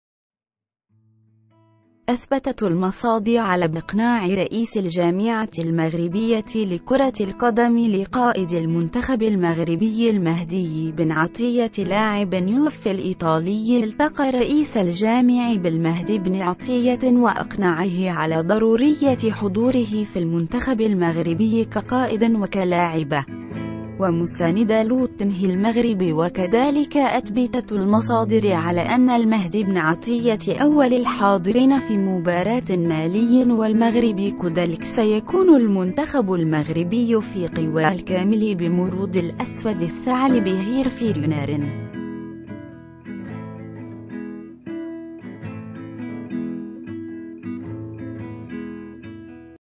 بنعطية سيكون اول الحاضرين في مباراة مالي
اقتناع بنعطية بكلام رئيس الجامعة فوزي